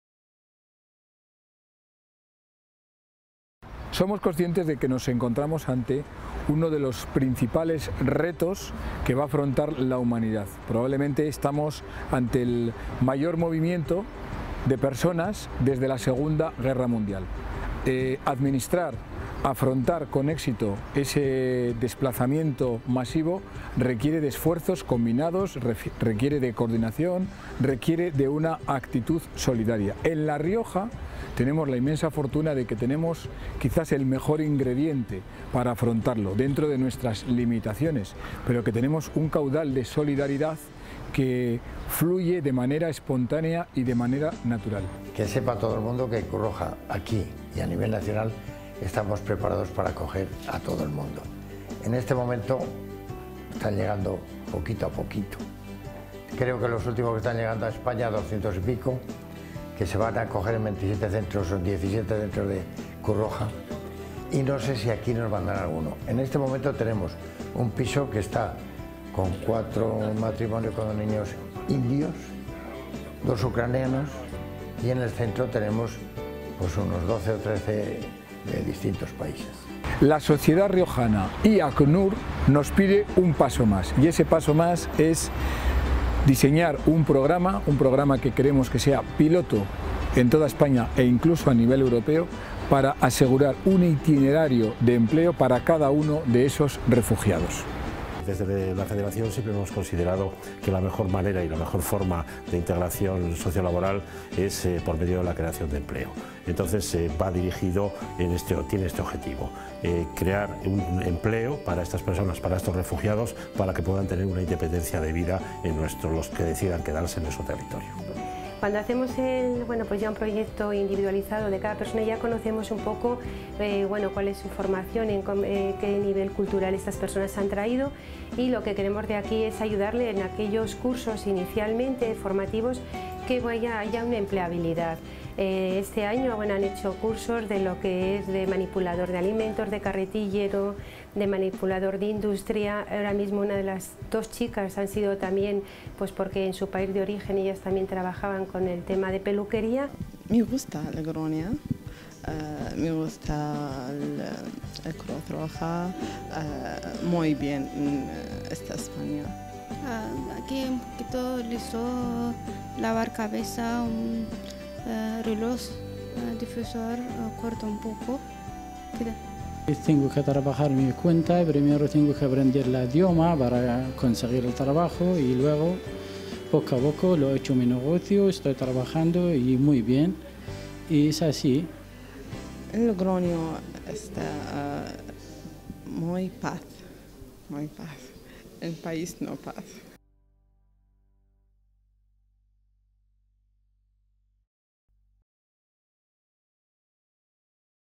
Día Mundial del Refugiado
El consejero de Políticas Sociales, Conrado Escobar, presenta el Proyecto de integración laboral de refugiados, invitado por ACNUR, el Día Mundial del Refugiado